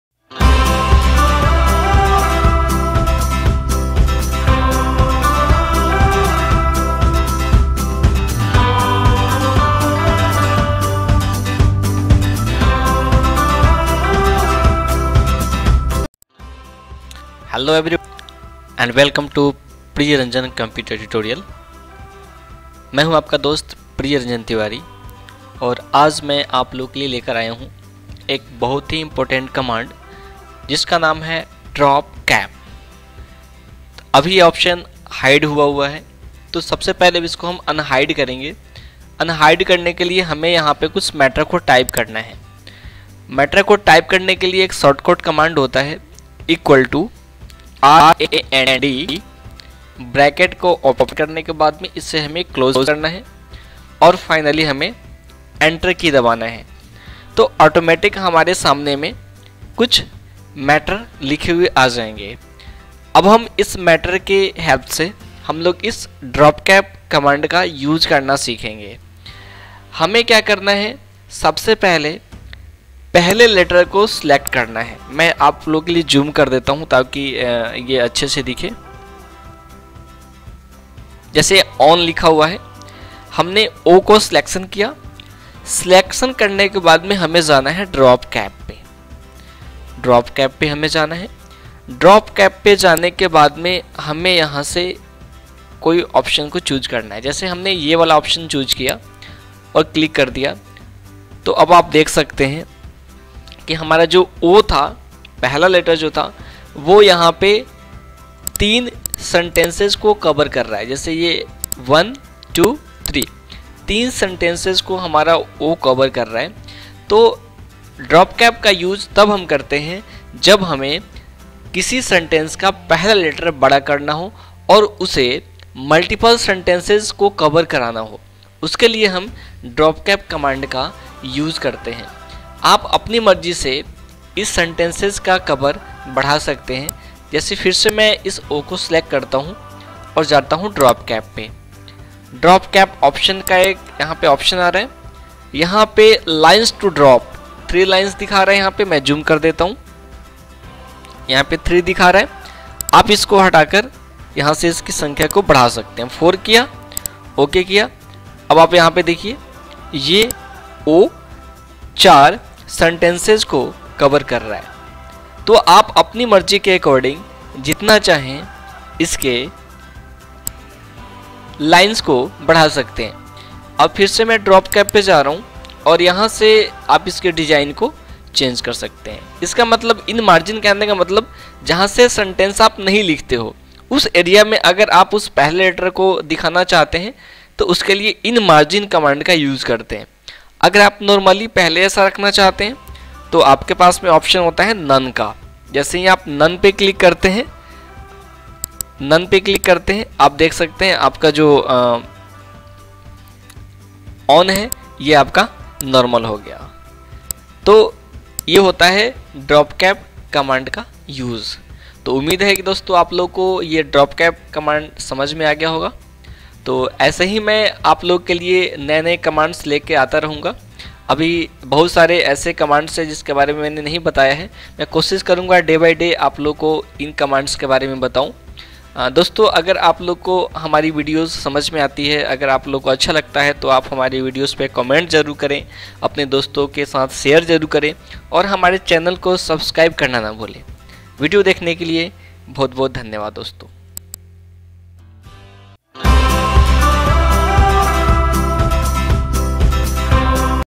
Drop Cap Command in MS WORD 2007 in Hindi ! ड्रॉप कैप कमांड का प्रयोग हिंदी में
Drop caps are easy to apply in Word. First, select the first letter in the paragraph to which you want to add a drop cap. In the “Text” section of the “Insert” tab, click “Drop Cap” and select the type of drop cap you want to apply